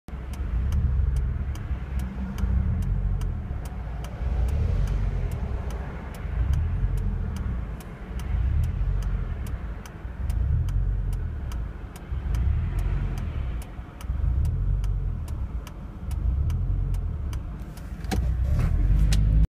MGF oscillating idle
Funny situation at the traffic lights on my Stepspeed MGF: After stopping the car and shifting into neutral, the idle keeps oscillating from 1,200 to 500 revs and back all the time.